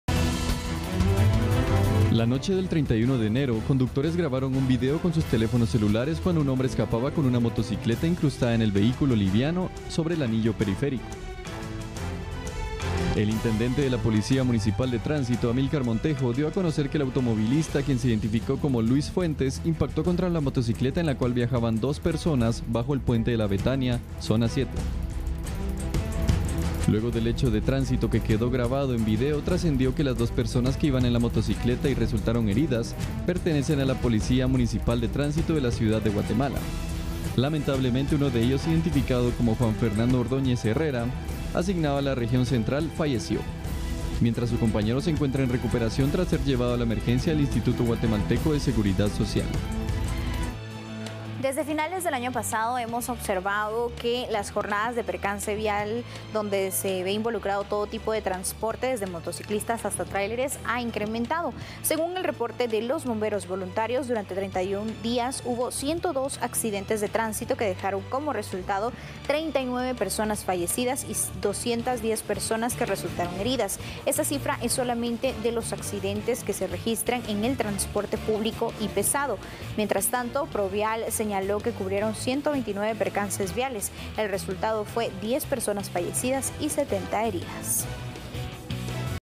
Un agente PMT fallecido y un herido por atropello
Según información preliminar, el conductor que los embistió se encontraba en presunto estado de ebriedad

Dos elementos de la Policía Municipal de Tránsito (PMT), fueron embestidos por un conductor en aparente estado de ebriedad.